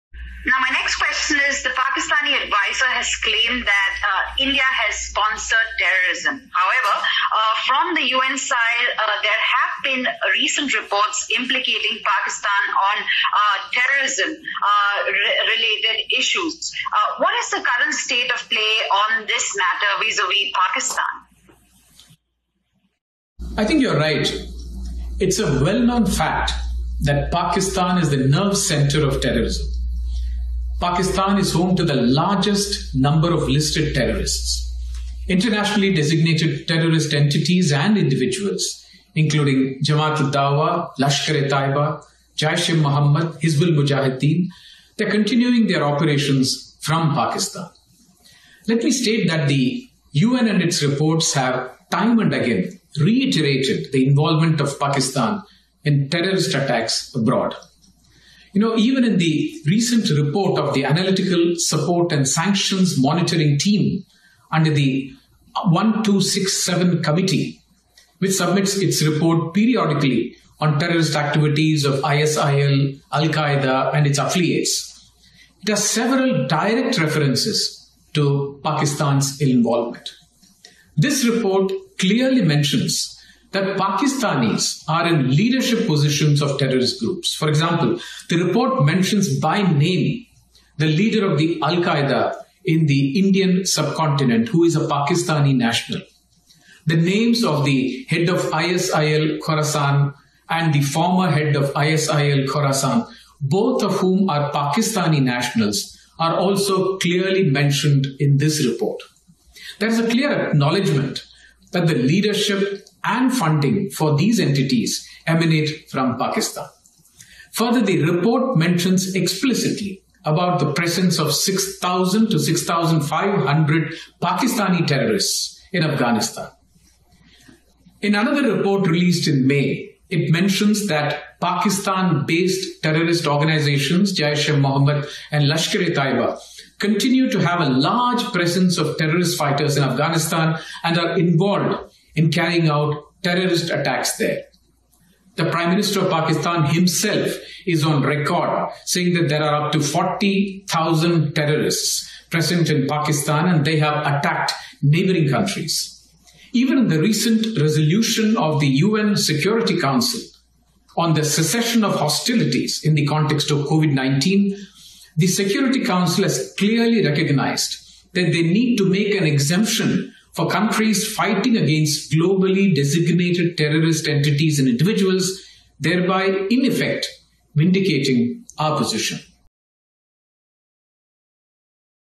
संयुक्त राष्ट्र में भारत के स्थायी प्रतिनिधि TS tirumurti ने Pakistan को आतंकवाद का केंद्र बताया
संयुक्त राष्ट्र में भारत के स्थायी प्रतिनिधि टीएस तिरुमूर्ति ने आतंकवाद को लेकर पाकिस्तान को एक बार फिर से आड़े हाथों लिया है। टीएस त्रिमूर्ति ने पाकिस्तान को आतंकवाद का केंद्र बताया और कहा कि  यह सर्वविदित है कि पाकिस्तान आतंकियों का गढ़ है। सबसे अधिक संख्या में लिस्टेड आतंकी का घर है पाकिस्तान। पाकिस्तान अंतरराष्ट्रीय स्तर पर आतंकी करार दिए गए जमात उद दावा, लश्कर-ए-तैयबा, हिज्बुल मुजाहिद्दीन और  जैश-ए-मोहम्मद के अलावा कई गुटों का ठिकाना है। संयुक्त राष्ट्र में भारत के स्थायी प्रतिनिधि टीएस तिरुमूर्ति ने संयुक्त राष्ट्र यानी यूएन की उस रिपोर्ट का हवाला दिया जिसमें विदेशी आतंकवादी हमलों में पाकिस्तान की भागीदारी को दोहराया गया है। उन्होंने कहा कि संयुक्त राष्ट्र ने अपनी रिपोर्ट में विदेश में आतंकवादी हमलों में पाक की भागीदारी को दोहराया। हालिया रिपोर्ट में, विश्लेषणात्मक सहायता और प्रतिबंधों की निगरानी करने वाली टीम जो आईएसआईएल, अल-कायदा की आतंकवादी गतिविधियों पर समय-समय पर अपनी रिपोर्ट प्रस्तुत करती है, इसमें पाक की भागीदारी के प्रत्यक्ष संदर्भ हैं।